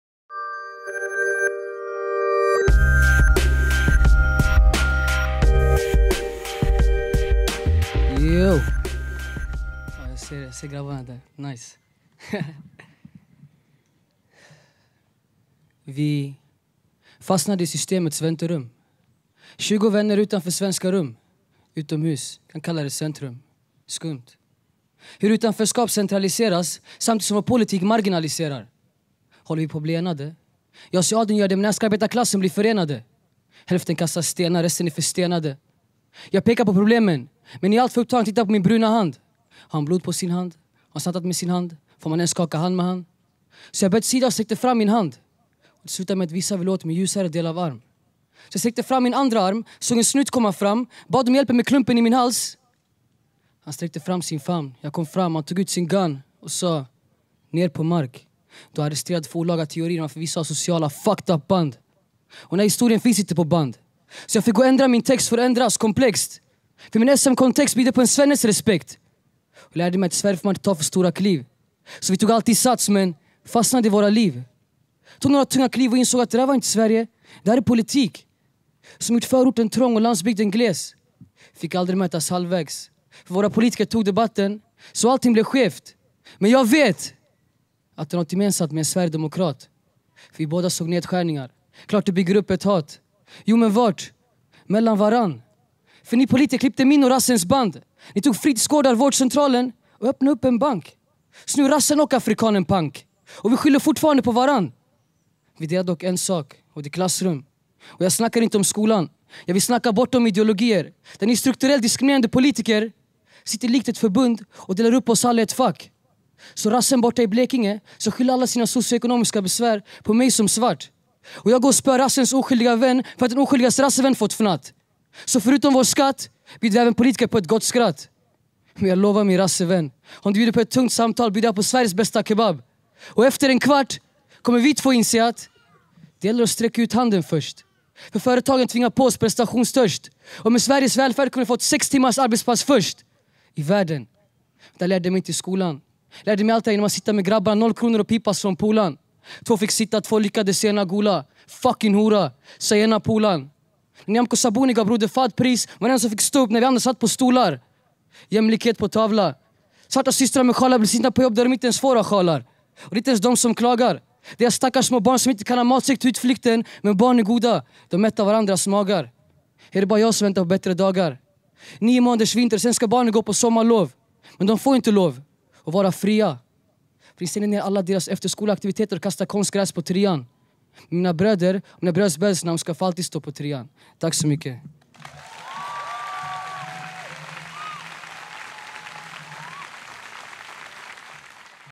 Simon - Systemets vänterum / Revolution Poetry @ Parkteatern 2016
Revolution Poetry på Parkteatern i Stockholm
Datum: 24 juli 2016
Film & klippning: Love Oijens